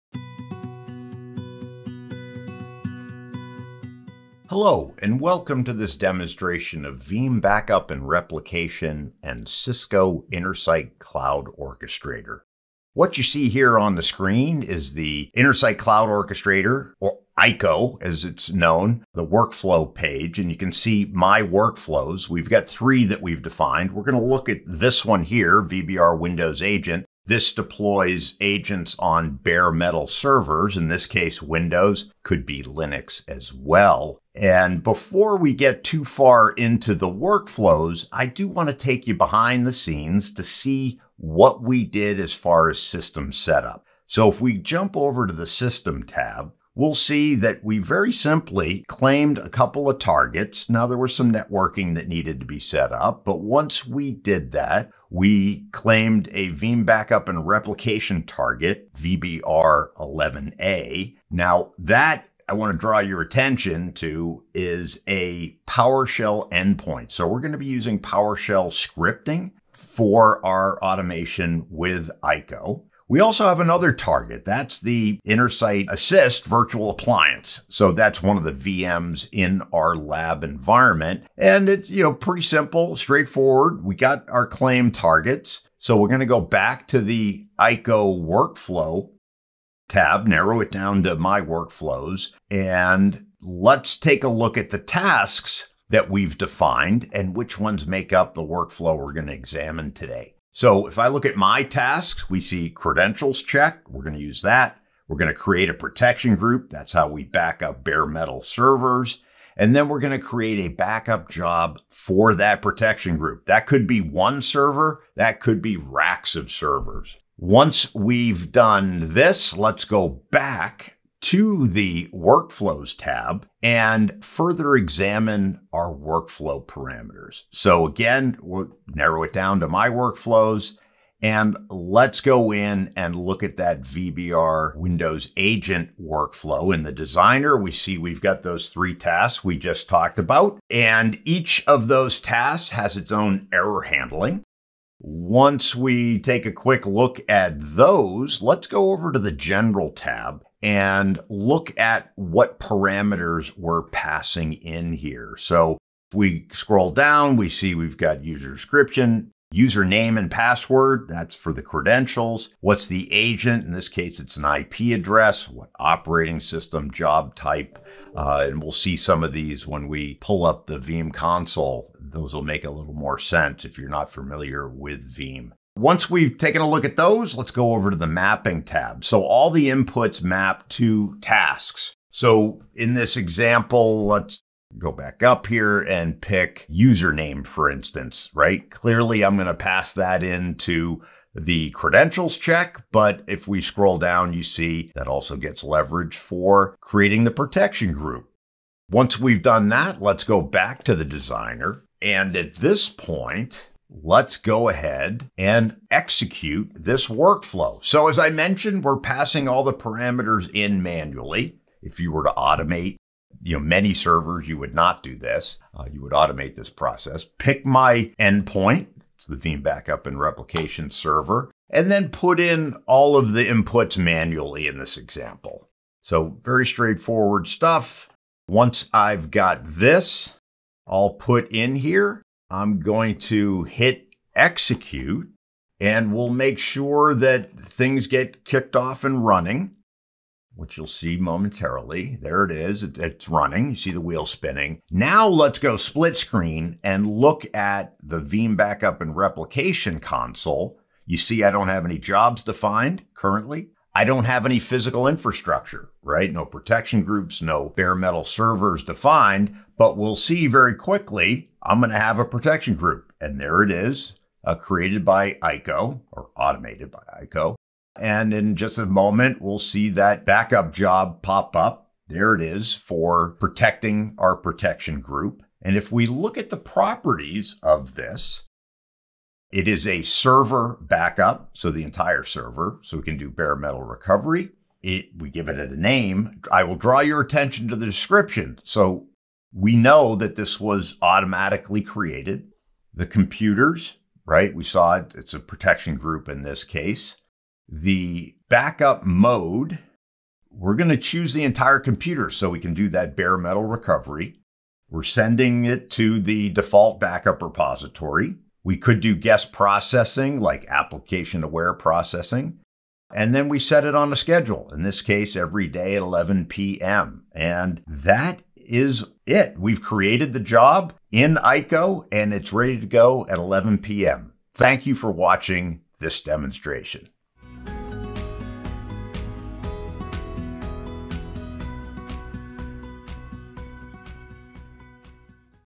Automating Veeam Backup with Cisco Intersight Orchestration
In this demonstration, you will see how Cisco Intersight Orchestration can automate and orchestrate a Veeam Backup & Replication workflow.  This demonstration shows the use case of automating the deployment of a Veeam Agent, then creating and scheduling a Veeam backup job for a newly added physical Cisco UCS Server.